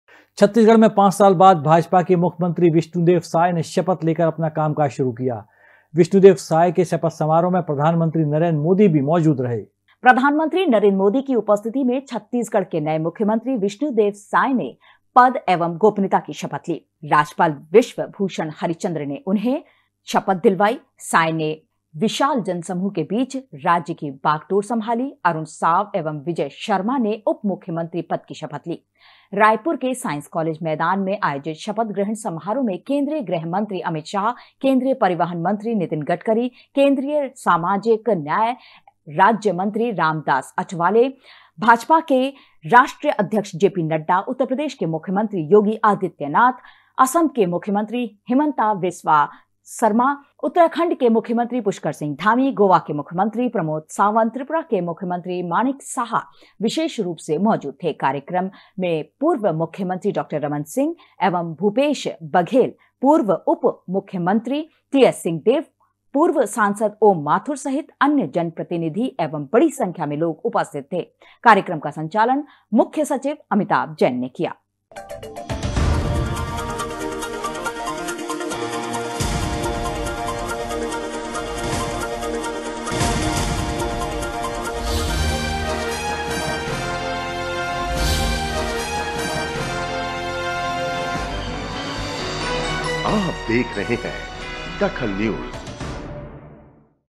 DAKHAL NEWS नरेंद्र मोदी के सामने विष्णु देव की शपथ
नरेंद्र मोदी के सामने विष्णु देव की शपथ
राज्यपाल विश्वभूषण हरिचंदन ने दिलाई शपथ